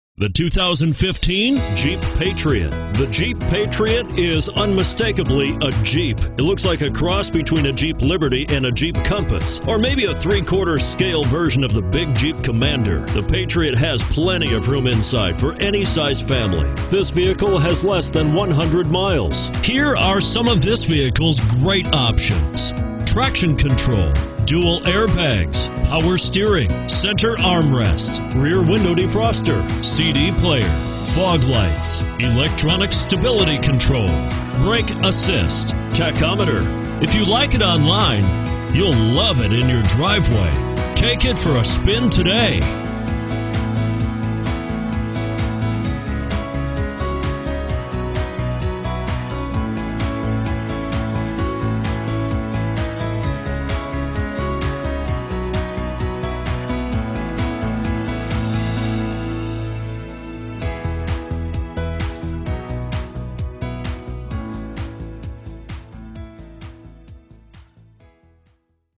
2015 Jeep Patriot David Stanley Dodge JP26647
2015 Jeep Patriot Sport

Patriot Sport, 4D Sport Utility, 2.0L I4 DOHC 16V Dual VVT, 5-Speed Manual, FWD, and Black. Best color! Drive this home today! Are you interested in a simply great SUV? Then take a look at this terrific-looking 2015 Jeep Patriot. When H2O starts showing up in the weather forecast, the FWD power delivery will help keep you in control of things.